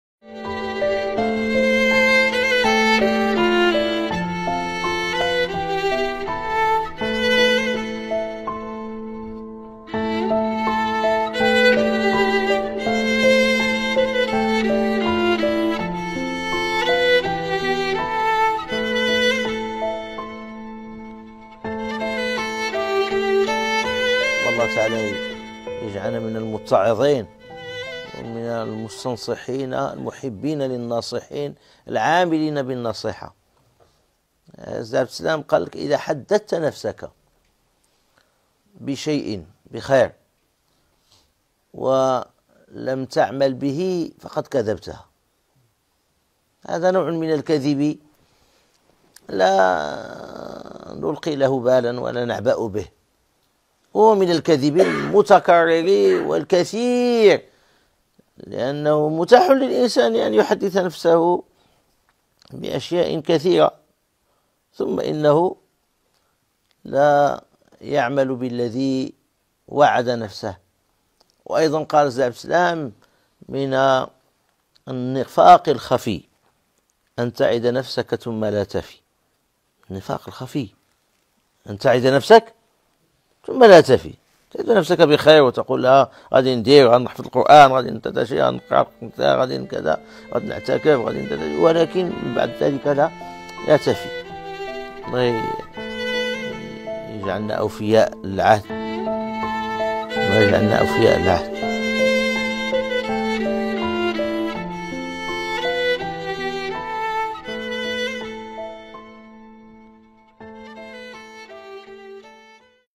الوفاء بالعهد
من  مجالس النصيحة
الوفاء بالعهد مع الأستاذ منير الركراكي
من النفاق الخفي أن تعد نفسك ولا تفي